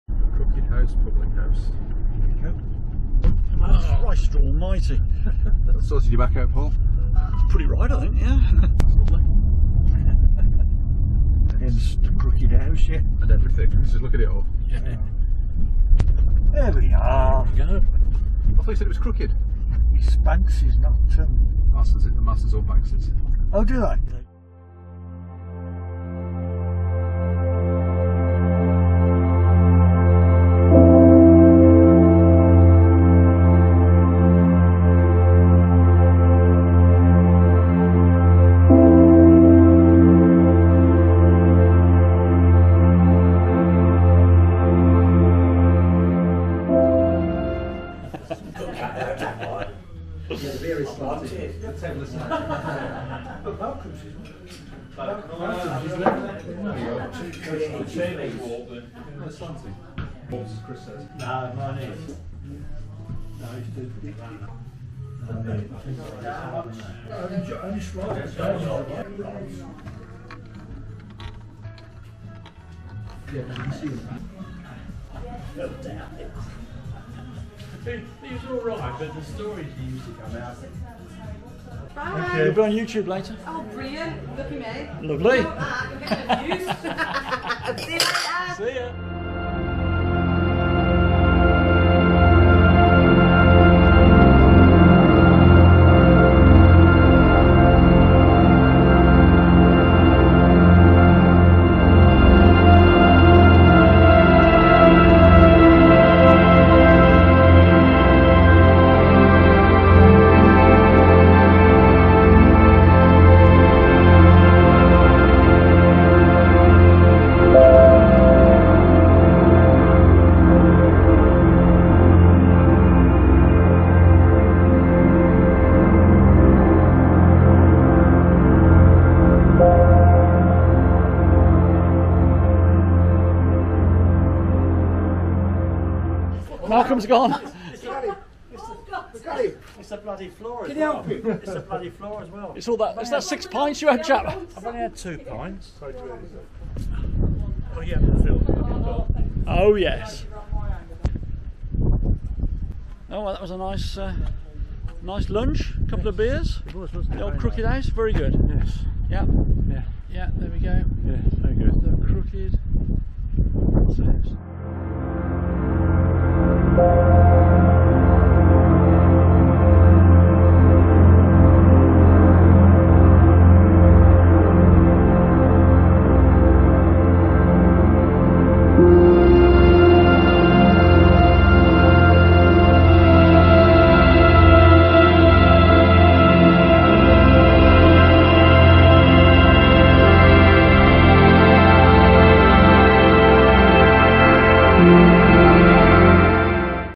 The Crooked House Pub.... Himley 👍
Lovely lunch and a couple of beers with the Cooper Close Lads. Malcolm, Chris and the two Daves.

Excellent food and very friendly staff 😎

The Crooked House is a pub and restaurant in South Staffordshire, England.

Its name and distinctive appearance are the result of 19th-century mining subsidence. One side of the building is now approximately 4 feet (1.2 m) lower than the other.

It stands in an isolated location close to Himley just off the B4176 road between Dudley and Telford, approximately 3 mi (4.8 km) from the nearest town centres of Sedgley and Dudley. The entrance to the road leading to it lies within the metropolitan county of the West Midlands, but the Crooked House is located just north of the boundary between Staffordshire and the West Midlands.

History
The Crooked House was built in 1765 and was originally a farmhouse. During the early 19th century, mining in the area caused one side of the building to begin gradually sinking. It later (c. 1830) became a public house called the Siden House, siden meaning "crooked" in the local Black Country dialect. The building was later[when?] renamed the Glynne Arms after the local landowner, Sir Stephen Glynne, on whose land it stood.

The building was condemned as unsafe in the 1940s and was scheduled for demolition. Wolverhampton and Dudley Breweries purchased the pub and rescued it by making the structure safe using buttresses and girders to retain its lopsided appearance.

Optical illusions
The building's leaning walls give rise to optical illusions, as with a gravity hill. These include glasses slowly sliding across "level" tables and a marble appearing to roll uphill.